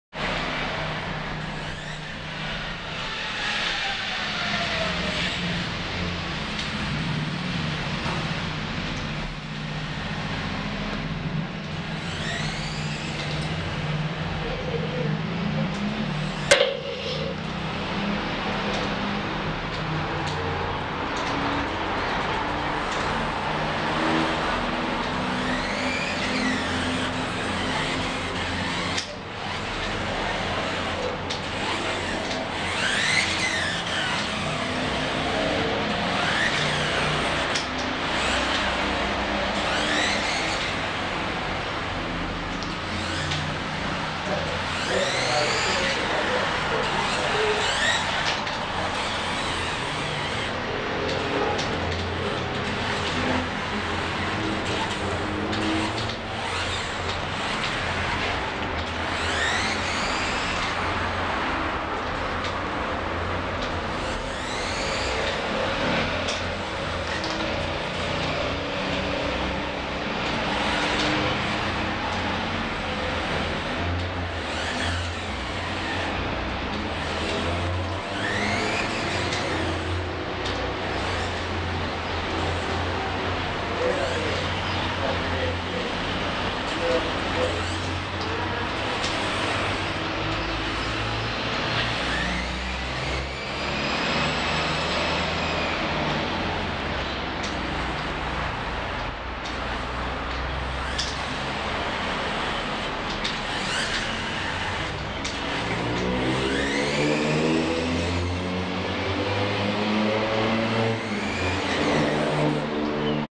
Radiocontrol Car - Prueba de manejo Chasis #2
RC CAR - Radiocontrol - Prueba de manejo Chasis #2

Prueba de manejo de mi chasis número 2. Que esta hecho con 4 motorreductores pequeños que funcionan para la tracción y dirección, dependiendo de cuales se activen, las llantas son de hule y los rines están hechos de estireno.